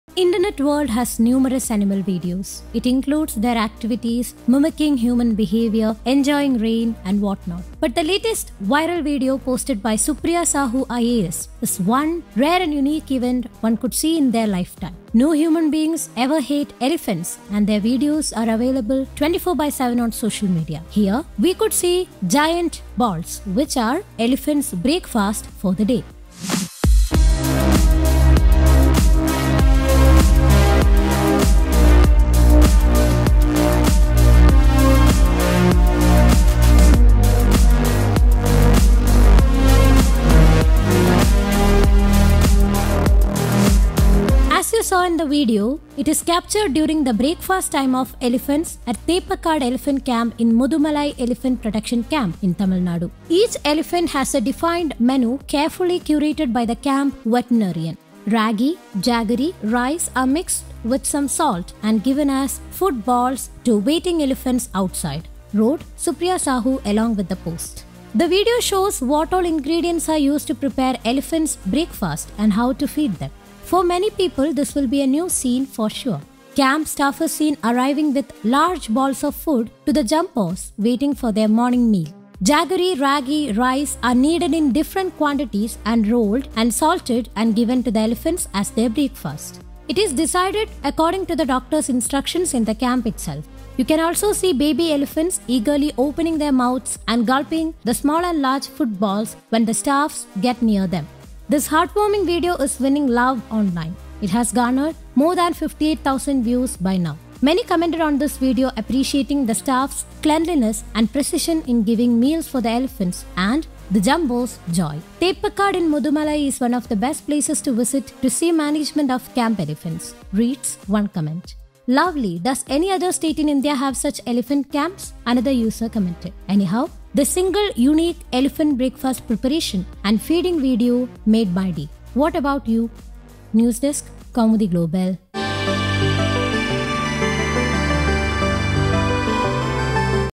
This is how people make breakfast for Jumbos at Tamil Nadu!
Internet world has numerous animal videos. It ranges from their activities, mimicking human behaviour, enjoying rain and what not. But the latest viral video posted by Supriya Sahu IAS is one rare and unique event one could see in their lifetime. No human beings hate elephants and their videos are available 24*7 on social media. Here, we could see giant balls, which are elephant’s breakfast for the day